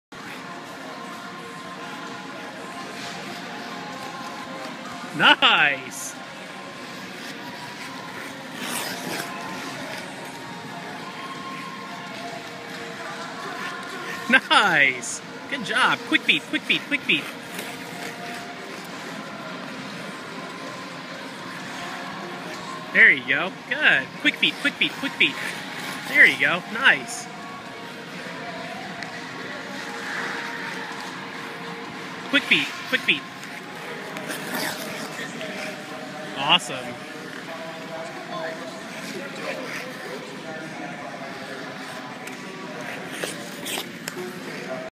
Six year-old hockey player, learning to skate. Quick backw
Just over two years on the ice.  Comfortable doing backward and forward crossovers, now learning to do them quickly with a little hop in his step.